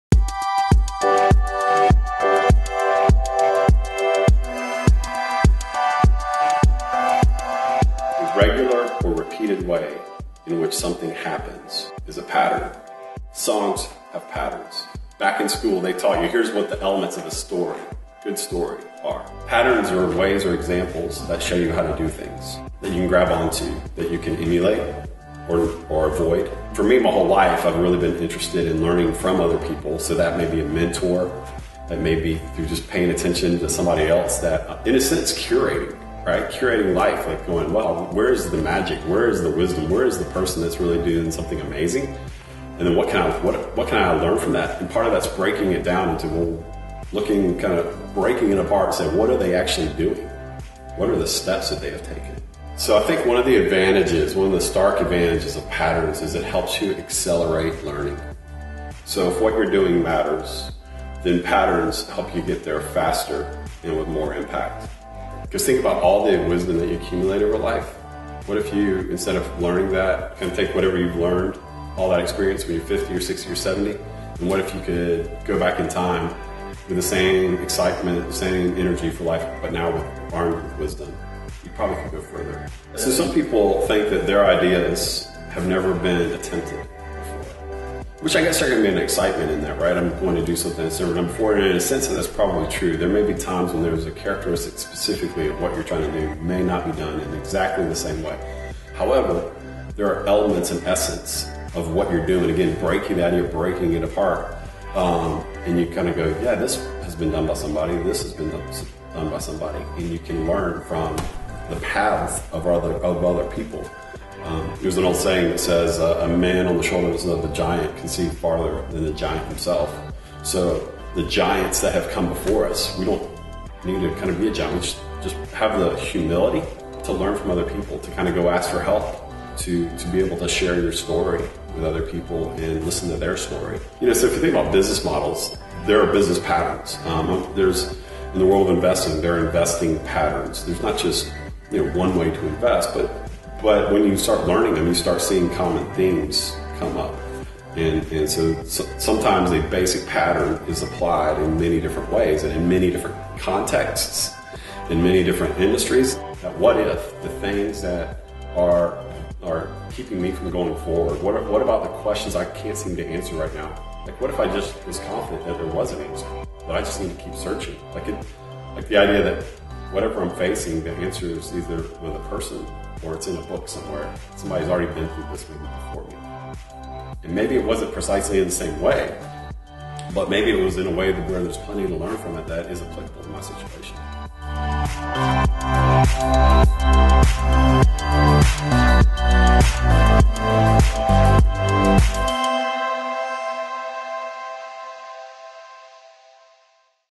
PATTERNS WITH GREG GILBERT
Plywood Presents is a gathering of problems solvers. This year we will craft learning through three distinct genres: Patterns, Rhythms and Brilliance. We have filmed interviews with thought leaders in our community that exemplify these distinct words. Our first interview is on the importance of Patterns. Greg Gilbert is a unique leader with  investment expertise and creative genius. He is a respected financial advisor and manages a team of 30 at Ronald Blue & Co., while also being the founder of a growing photographic community called My Athens. We had the opportunity to sit down with Greg to get a glimpse of how he sees patterns everywhere in society and what may happen if we see from the shoulders of the the giants walking before us.